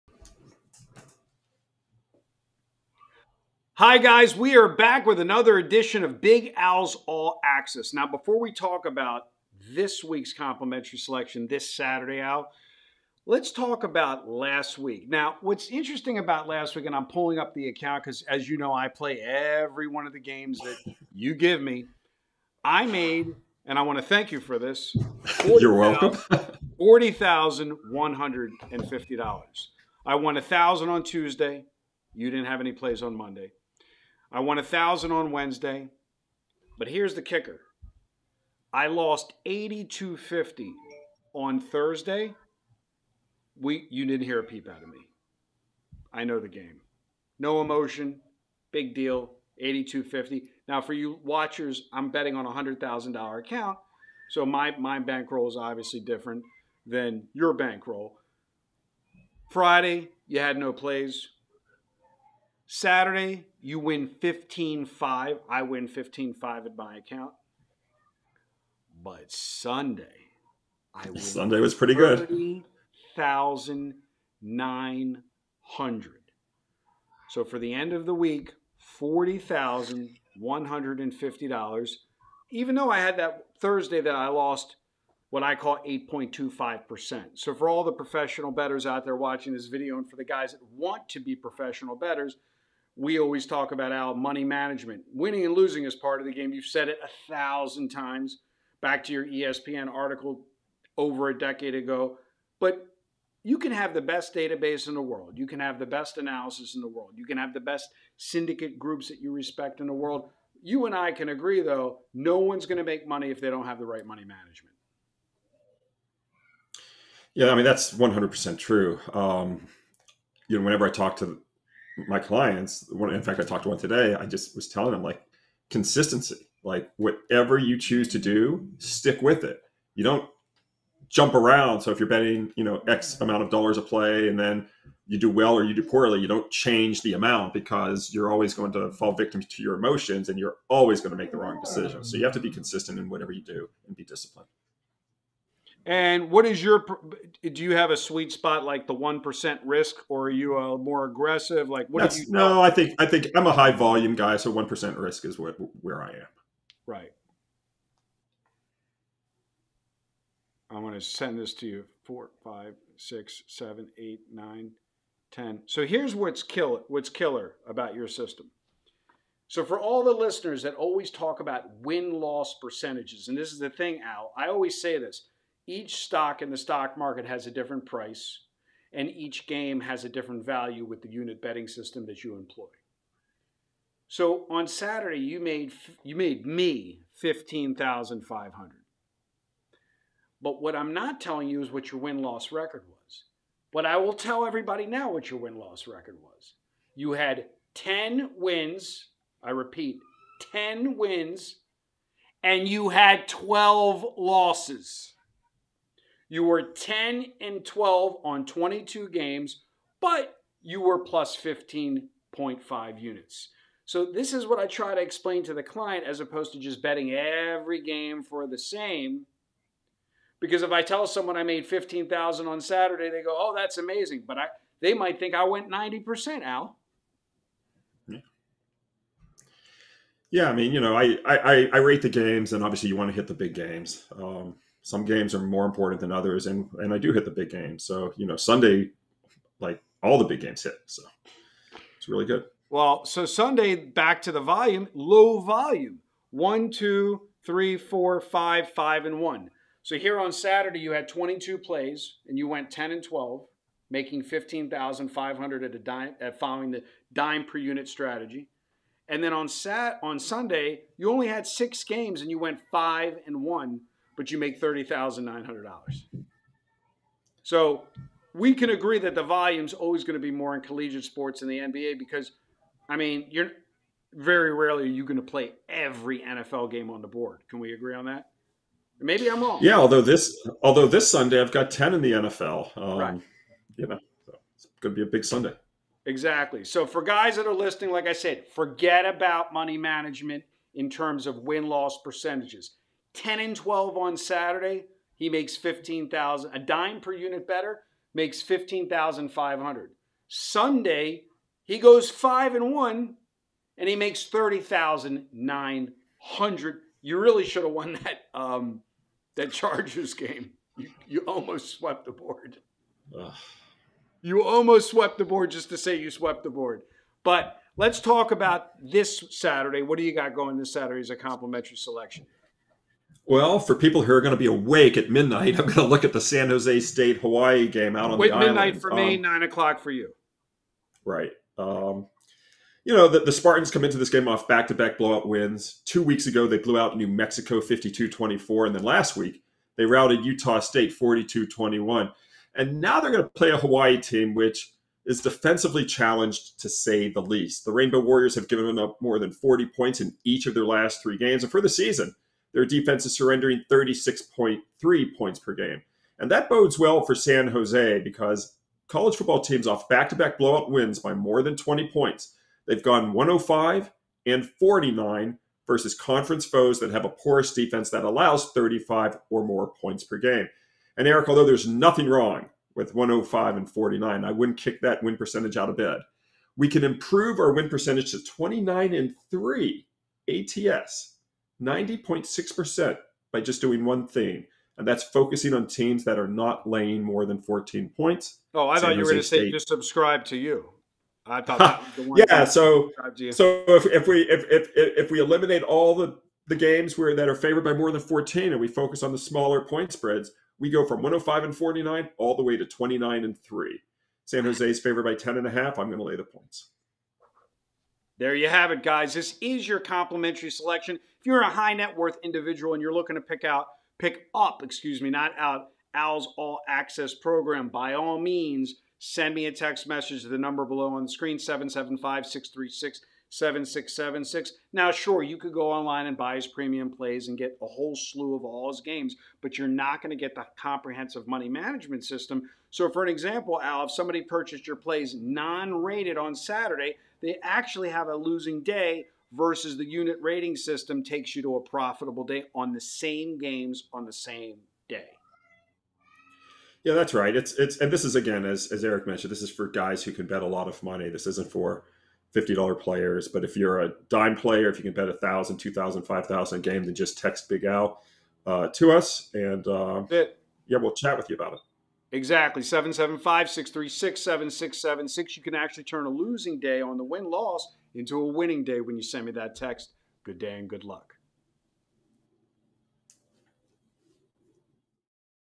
Saturday 10/28/23 College Football Pick and Prediction | ALL Access Big Al
Get the latest information from one of the best in the Business, the Legend Big Al McMordie  on this Saturday matchup as well as find out how to access the best sports betting database win the world!!!